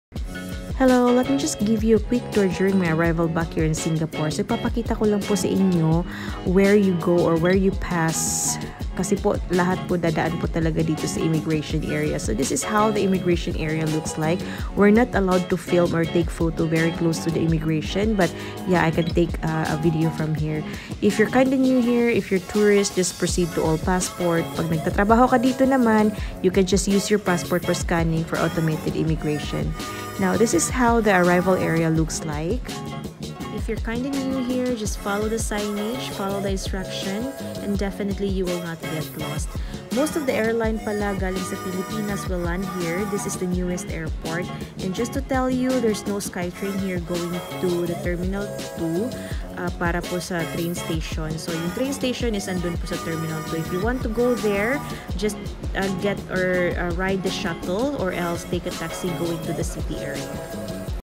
Walk-through at the ARRIVAL Area of CHANGI AIRPORT T4 | The newest airport of Singapore
Walk-through at the ARRIVAL Area and Hall of CHANGI AIRPORT T4 | The newest airport and the best airport in the world.